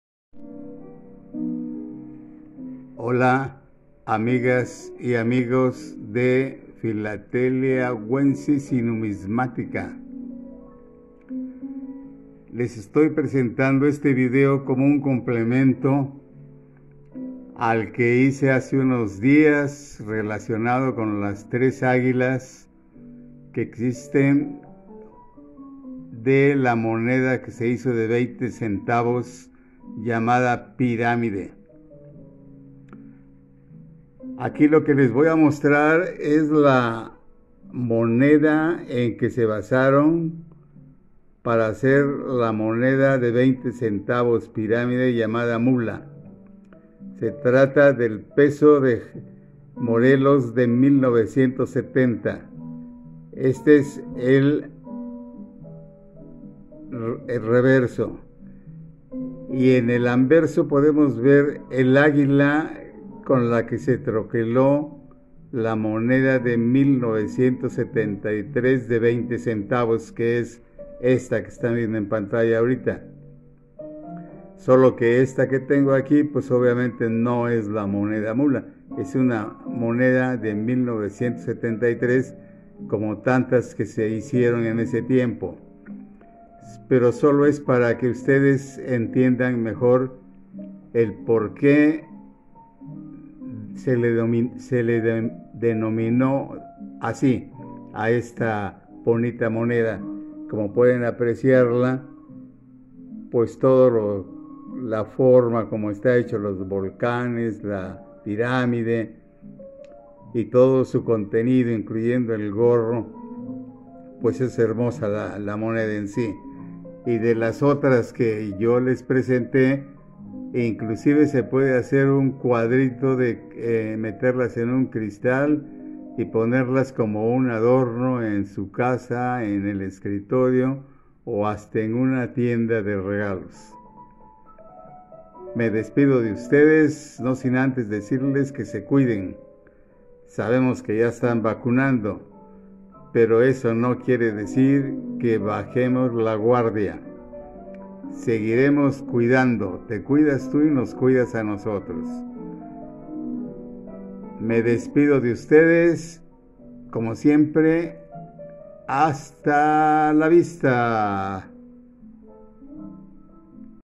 Video 28 Filatelia Wences y Numismática.
Este vídeo es un complemento al vídeo en donde presenté las tres águilas diferentes que estuvieron vigentes de 1943 a 1974, por lo que presento la moneda de 1p de Morelos de 1970 de donde se troquelo el águila de la moneda llamada "mula" de 1973.